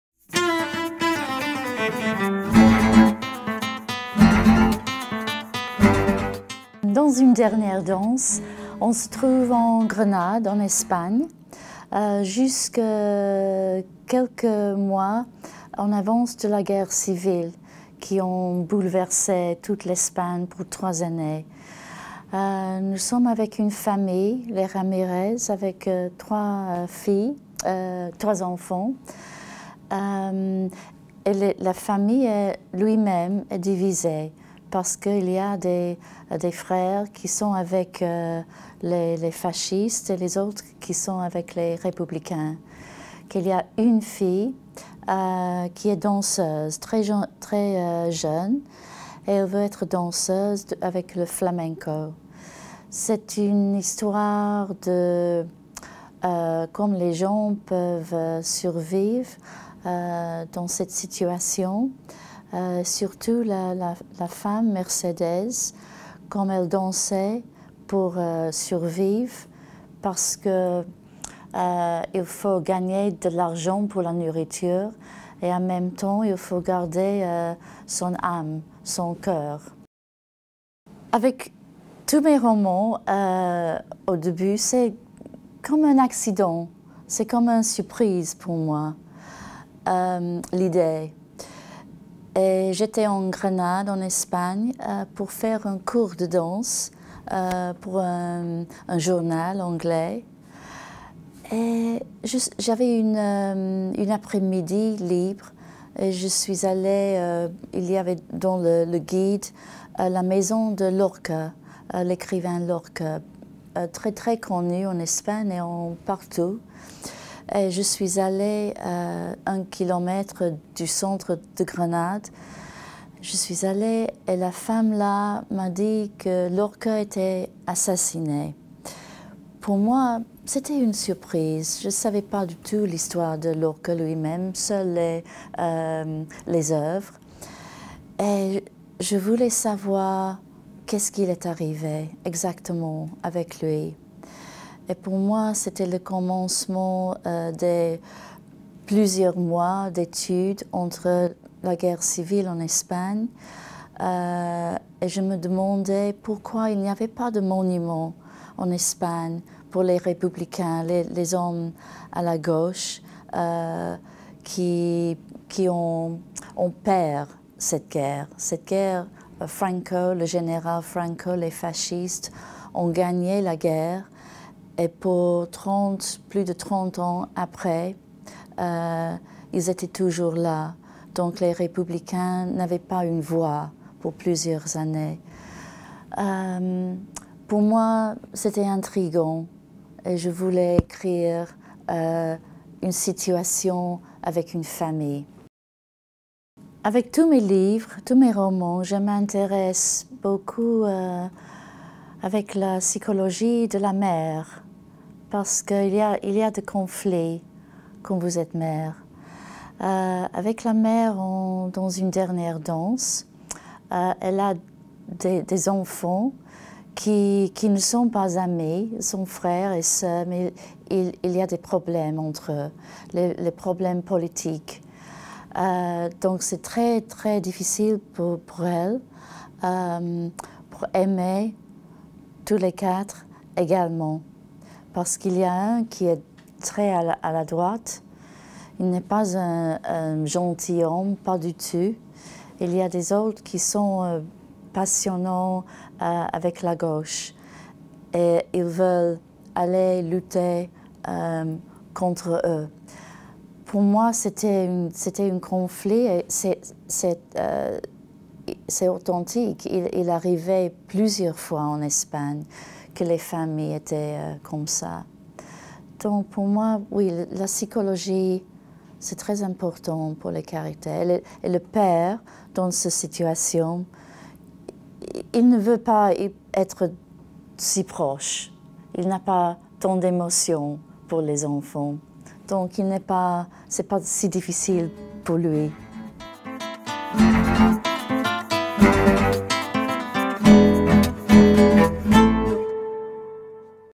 Victoria Hislop - Une dernière danse
Victoria Hislop vous présente son ouvrage "Une dernière danse aux éditions Le Livre de poche. Traduit de l'anglais par Séverine Quelet.